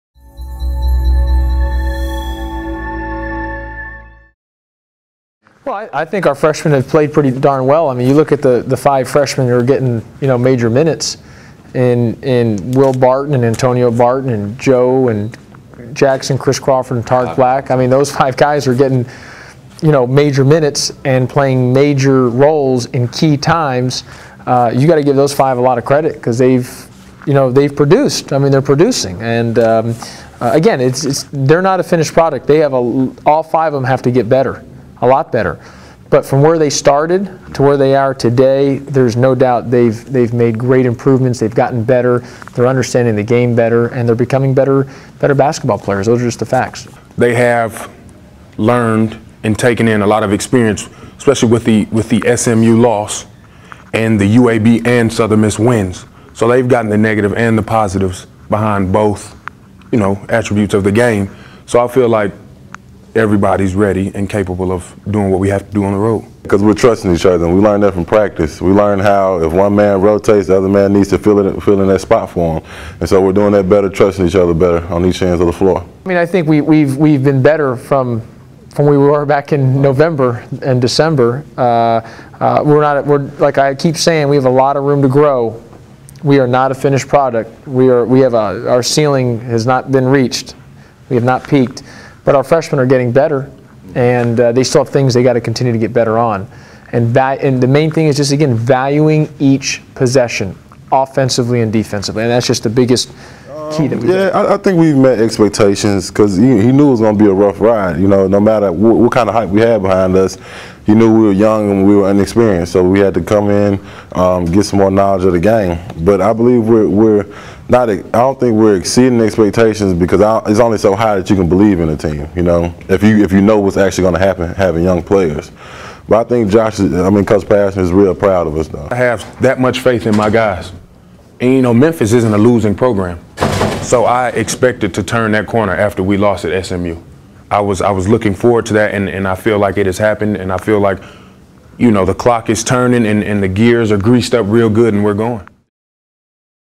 Freshman are getting better
After a win over UCF, Memphis coach Josh Pastner says his young team is showing improvements.